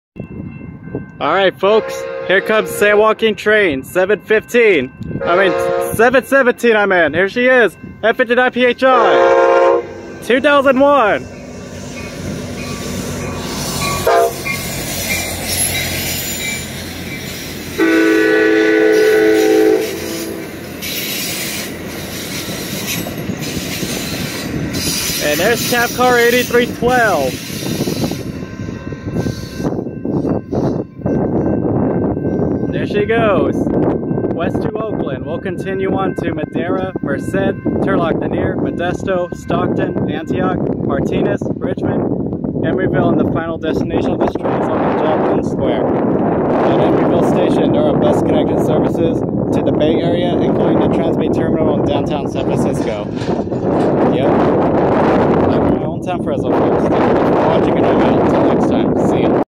Amtrak San Joaquins 717 passing by Church Ave in 4K 60FPS
Leading F59PHI CDTX 2001 and CabCar CDTX 8312 in the rear